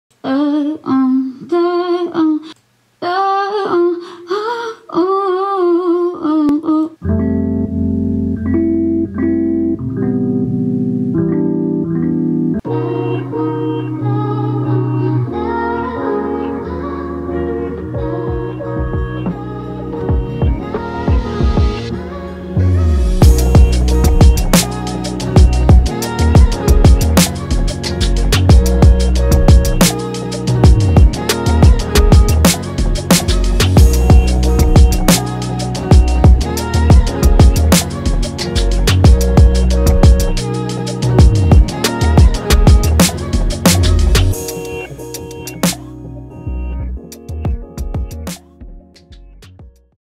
Making a J. Cole Type Beat 2018 "FREE" - Hip Hop Instrumental by DopeBoyzMuzic
Over 290+ Beats - All Leases Buy 1 Get 3 FREE!